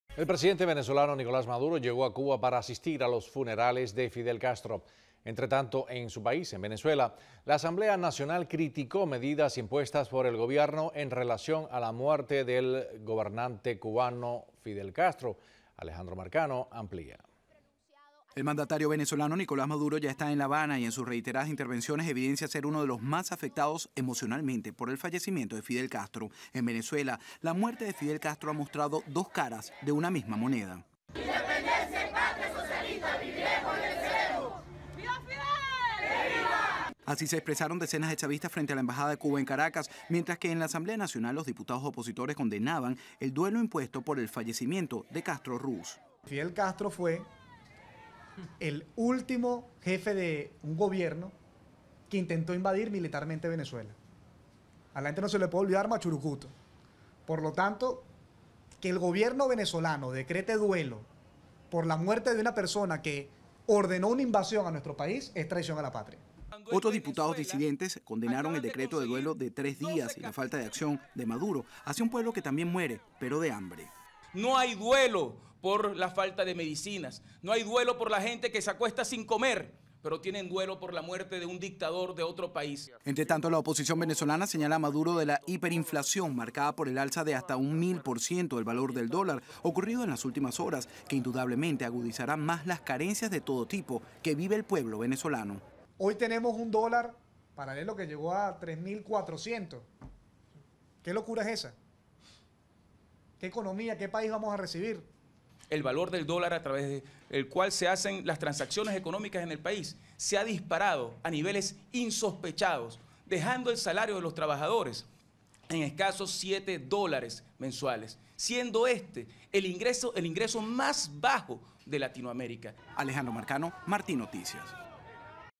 Asamblea Nacional de Venezuela critica medidas de duelo por Fidel Castro
El presidente venezolano Nicolás Maduro llegó a Cuba para asistir a los funerales de Fidel Castro. Entretanto, en Venezuela, la Asamblea Nacional criticó medidas impuestas por el gobierno por la muerte del gobernante cubano.